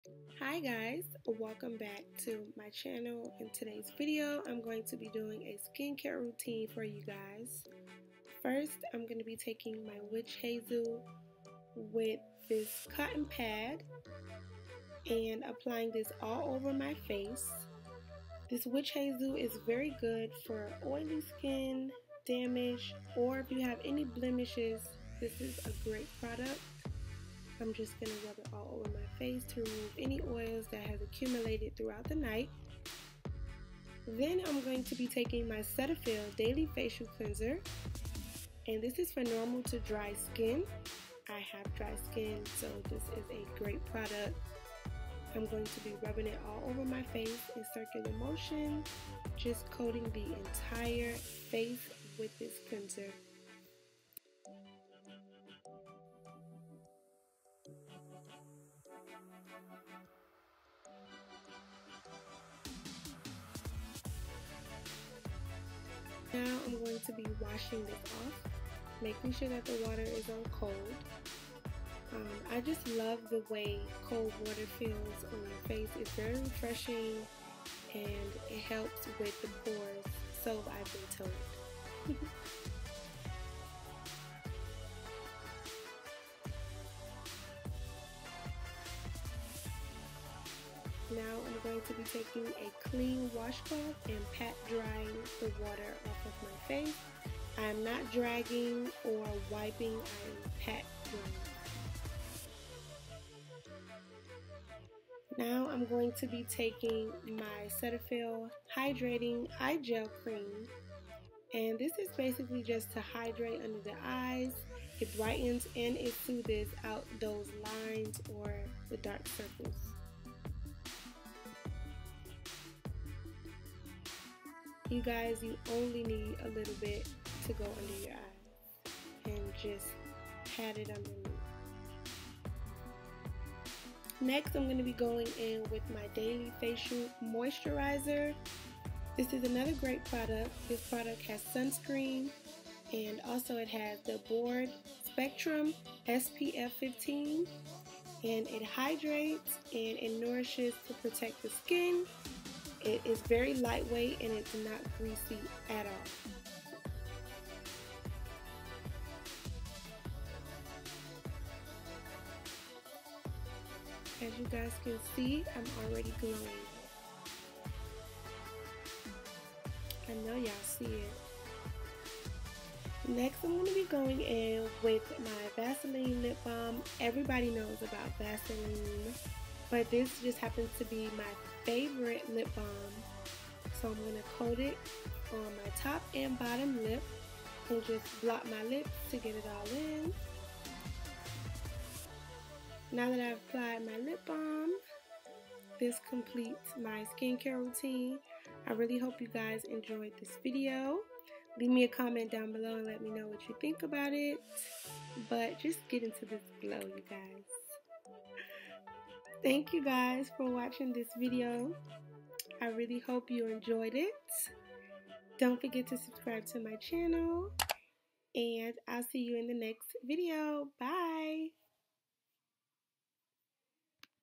SKINCARE ROUTINE | HOW TO GET CLEAR/GLOWY SKIN | LifeAsLilLori
Hi Loves, Here is my everyday SkinCare Routine. Remember these products are what works best for my skin, so just know it may or may not work for you. I hope you guys find it helpful and enjoy!